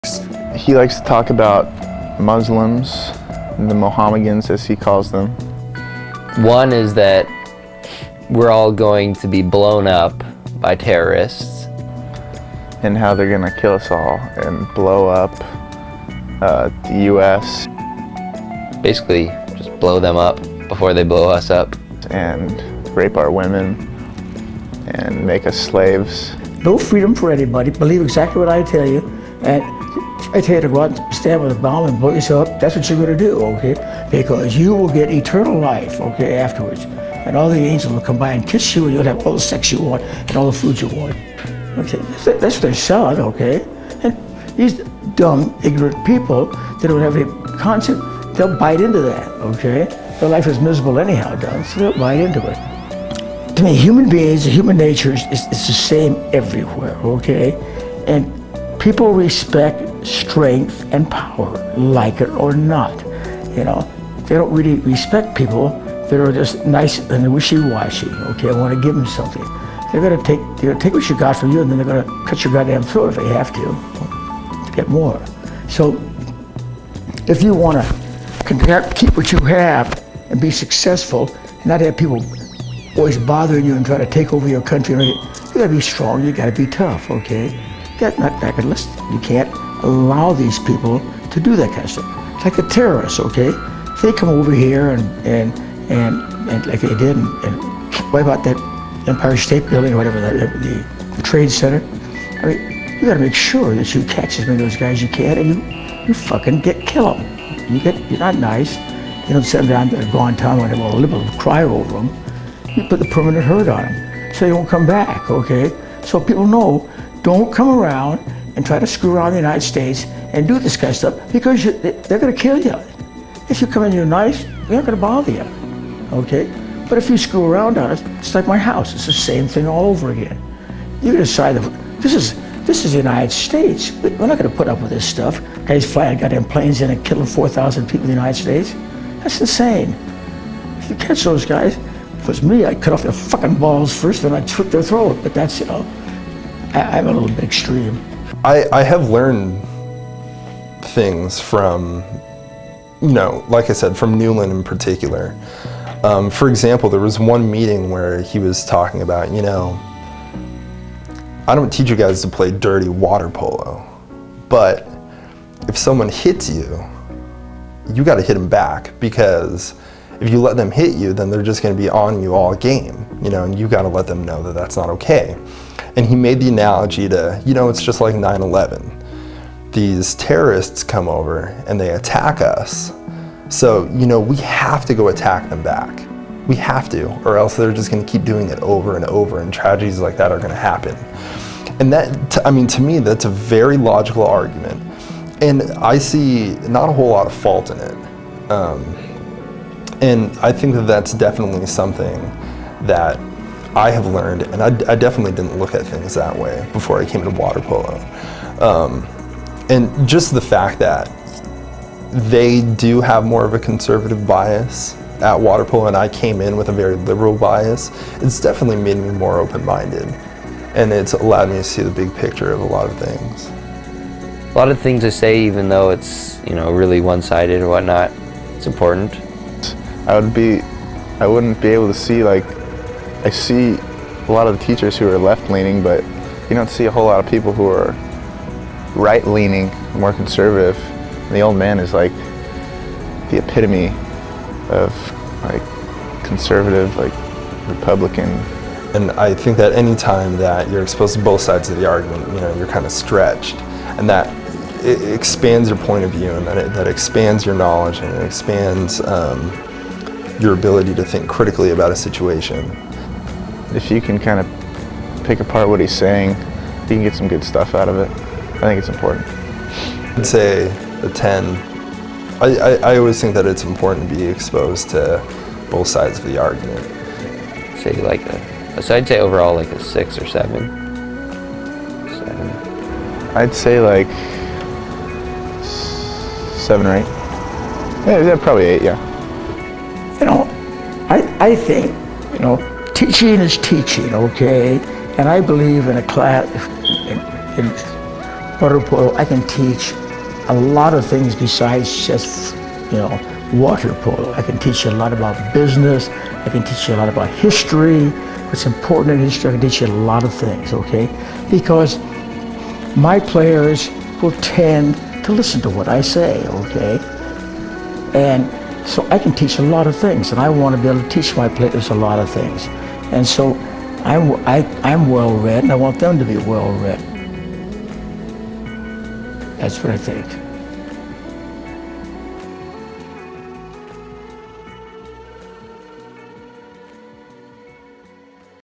Documentary - Politics and Polo - Part 2
This is a video starring UCI water Polo Players and Coach Emeritus Edward Newland.  The video appraises how teachers discuss topics seemly unrelated to their area of expertize.  The video details how teachers use their place of authority to interject personal values and beliefs.  In Newland's case, he is no more guilty than the left-winged university professors, grading papers based on what they feel to be correct opinion.  In the end his voice, no matter how extreme, is essential in facilitating holistic growth for young men in an academic environment.